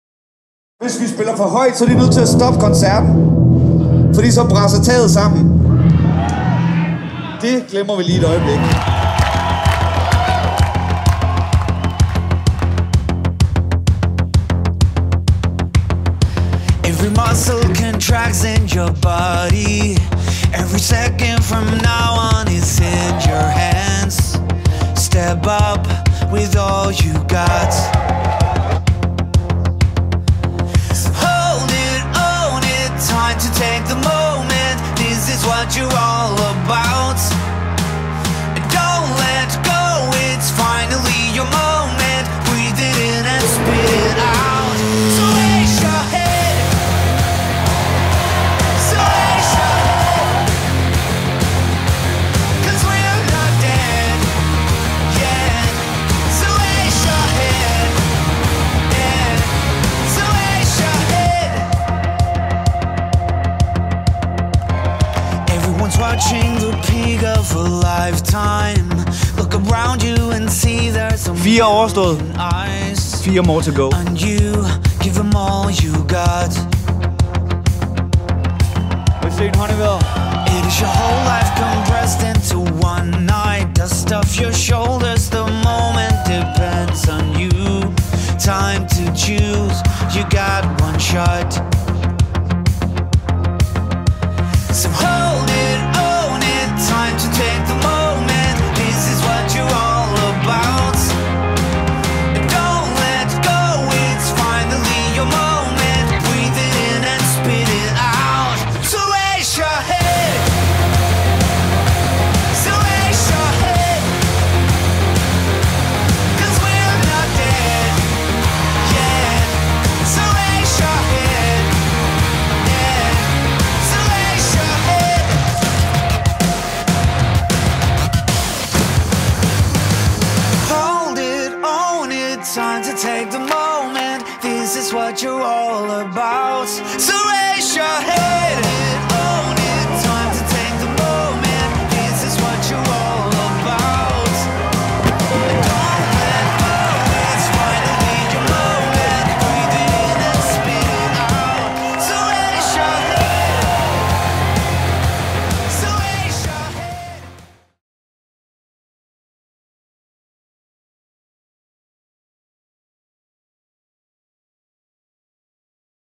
TAK ROCK TIL ALLE!
Verdens hurtigste Danmarksturné med TAK ROCK! og Carpark North